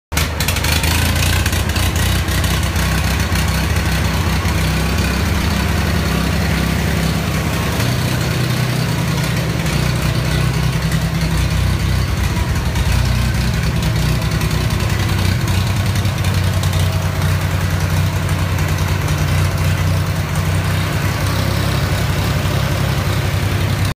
Rough running Ford 3000 gas tractor!
What's best here? Spark plugs, fuel additives, carb cleanup?